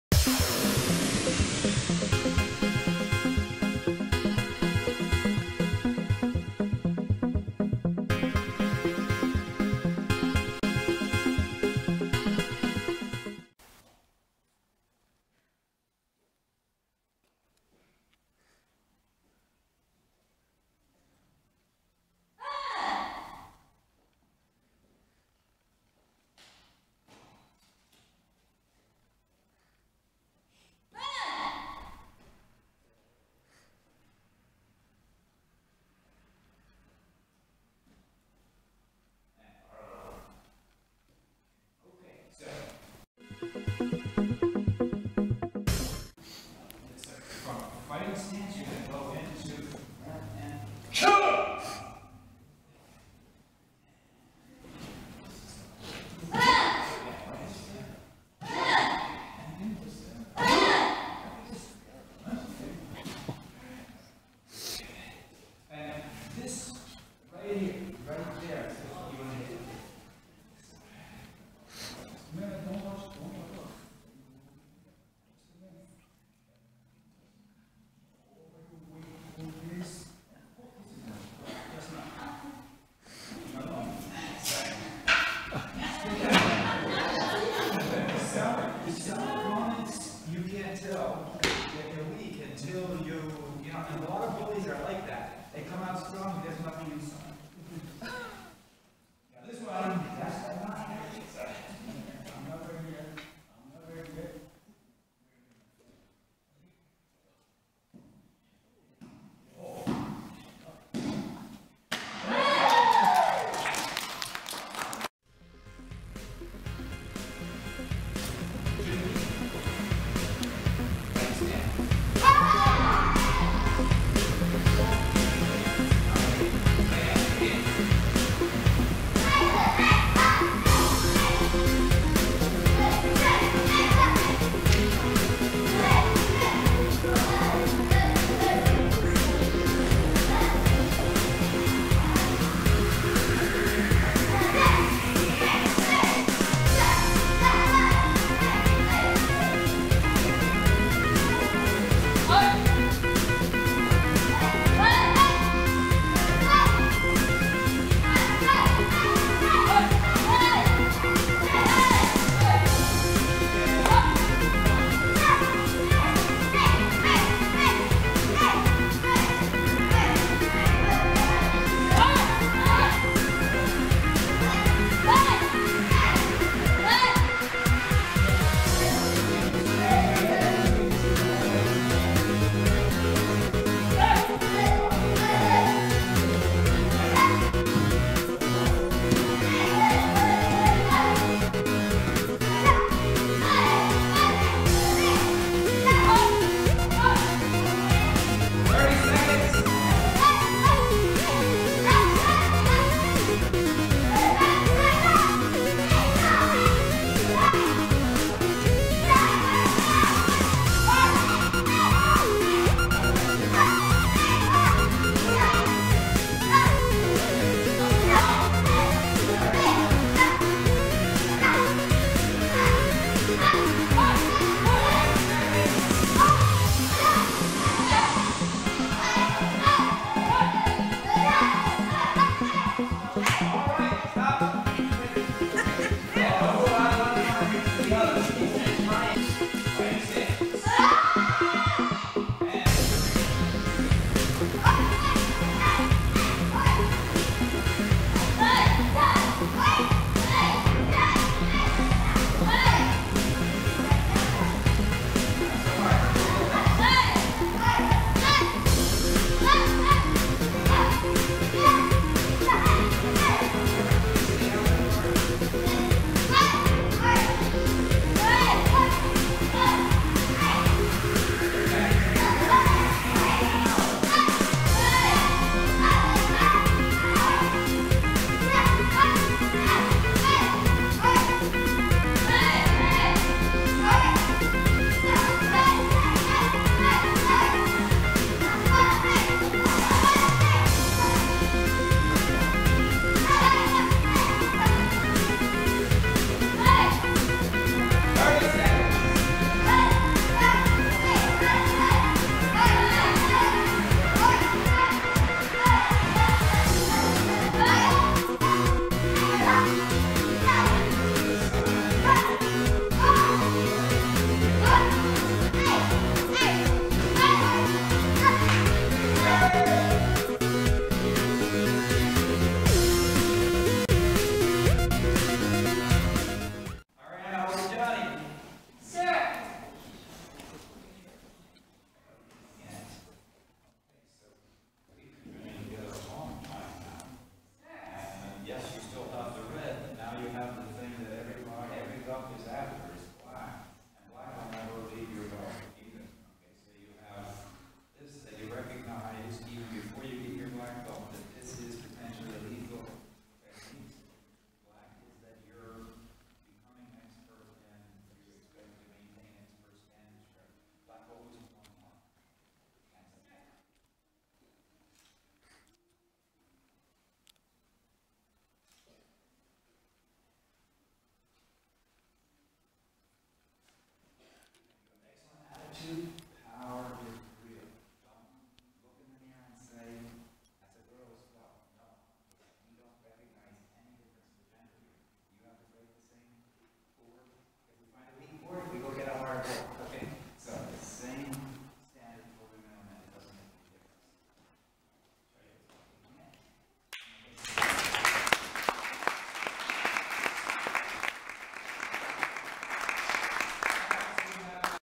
Aryana's Cho Dan Bo Test
This is her last gup test, next is two tests for 1st degree black (may not be recorded).